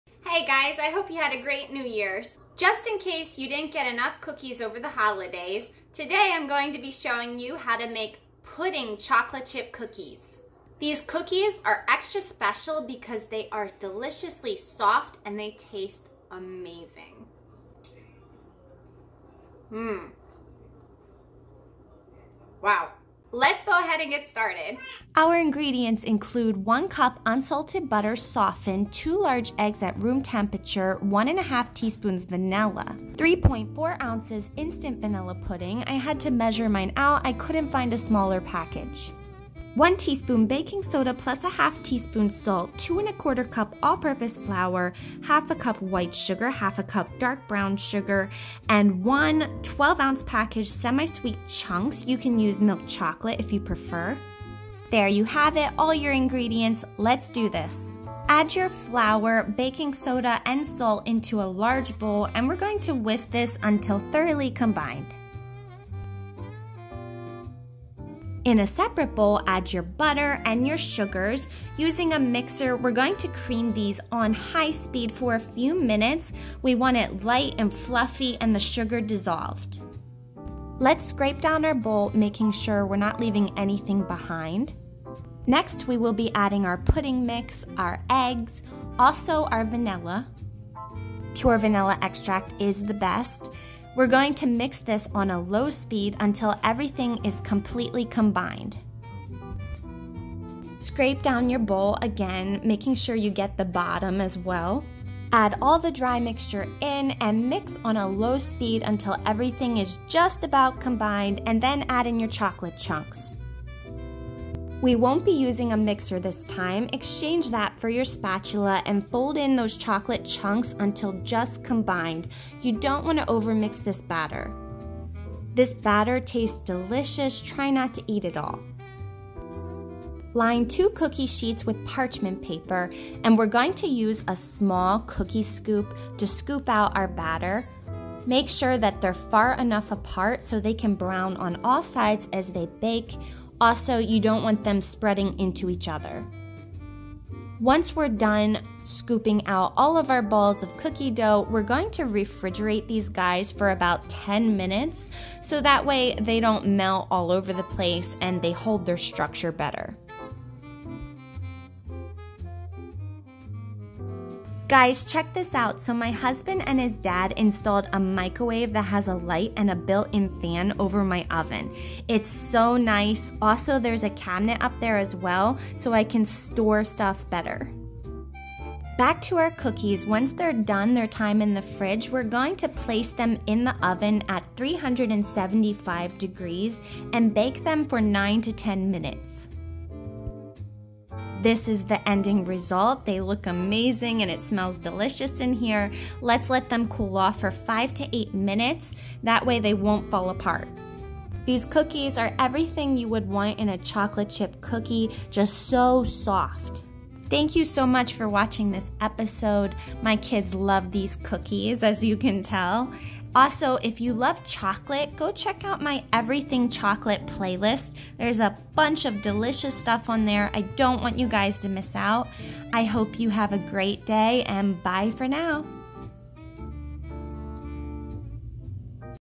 The BEST Pudding Chocolate Chip Cookies
This recipe make the SOFTEST Chocolate Chip Cookies that taste like the perfect Chocolate Chip Cookie!!